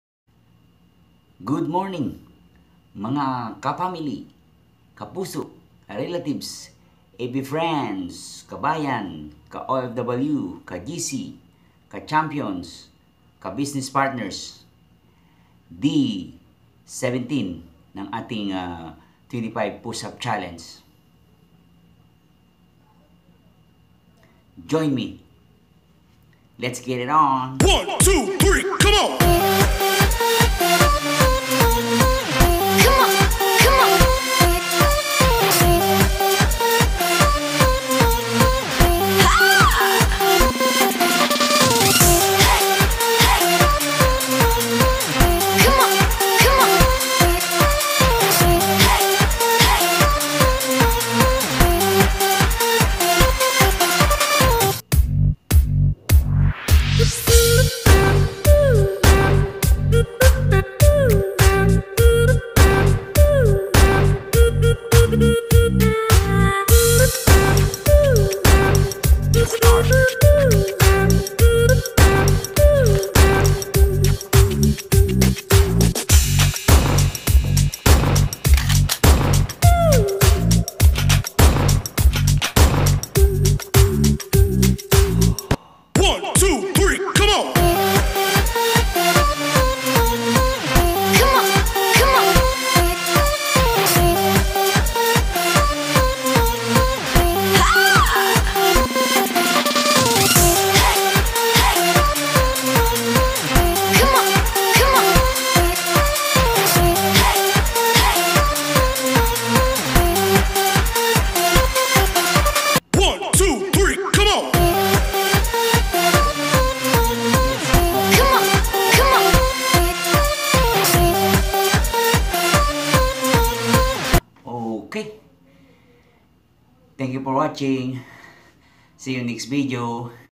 Day #17: 25 Push-Ups Challenge, Awareness for (PTSD) Post Traumatic Stress Disorder.
Hi Guys!

I've been nominated to do 25 push-ups challenge, to hopefully raise awareness for ( PTSD ) Post Traumatic Stress Disorder and Depression.

Today, I nominate you!!! I'll do hope you do also.

The Rules are Simple:

*Once you are nominated, 25 days starts the following day.

*Everyday you record yourself doing 25 push-ups even if you have to drop your knees to get 25.

*Everyday you must nominate a different person.

*God Blessed!